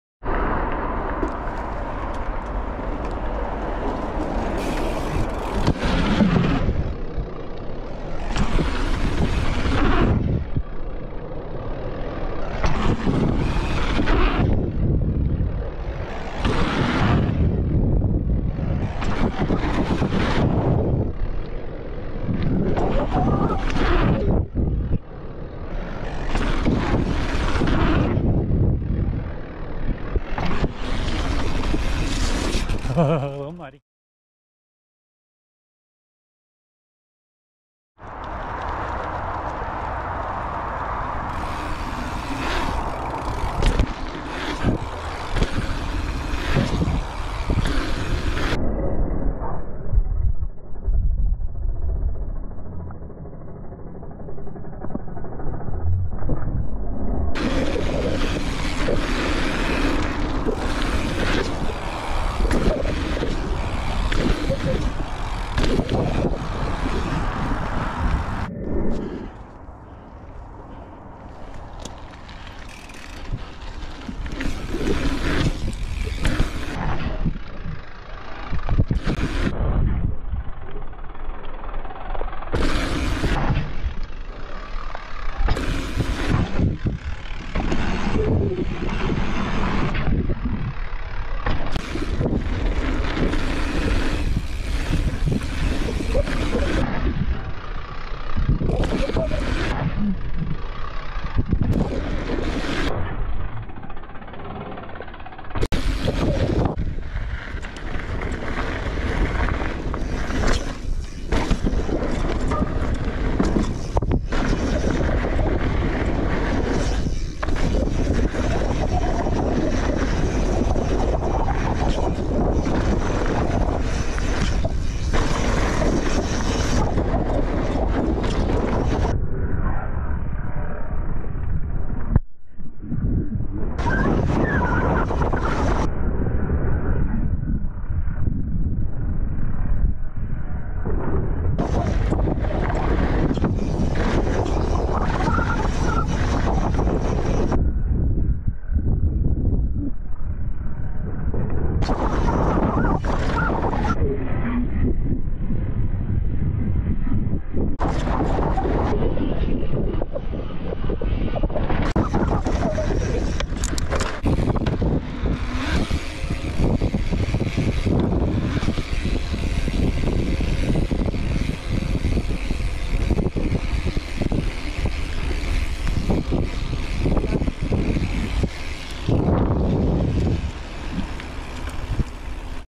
Sierra Vista Bike Park Reno
First time hitting all the jumps on the Medium Slopestyle Line @truckeebikepark2227 .  Jaws is always a guaranteed good time.  Barbara Jean and SNP were both closed for maintenance so I hit the little line in the lower DJZ.  A small variation on the Advanced line at Sierra Vista Bike Park in Reno. Hipping off the last jump to the right and trying to extend it as far as possible landing on the sloped edge of the trail.            It was a good day!